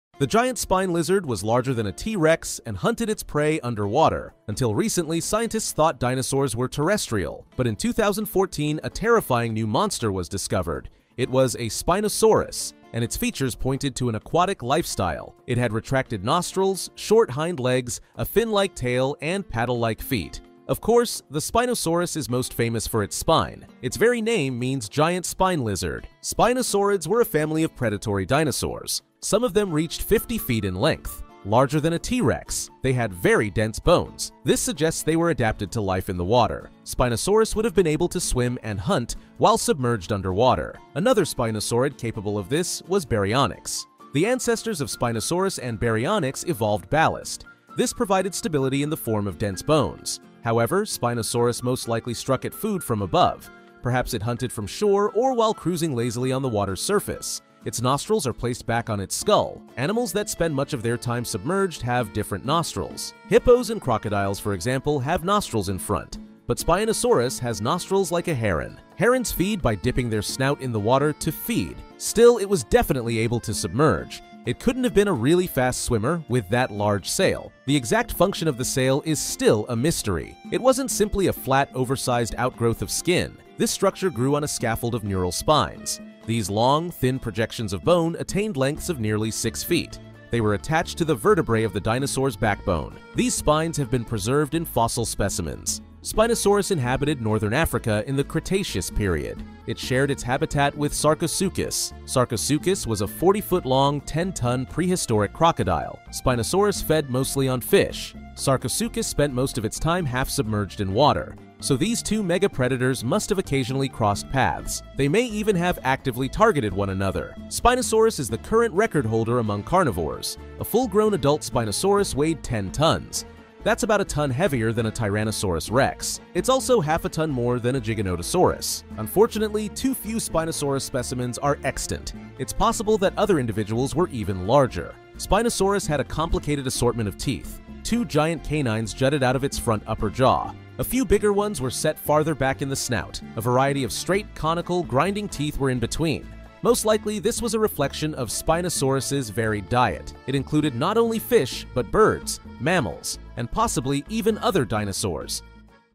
The Giant Spine Lizard Was Larger Than a T. Rex & Hunted Its Prey Underwater
The largest carnivorous dinosaur was a swimmer!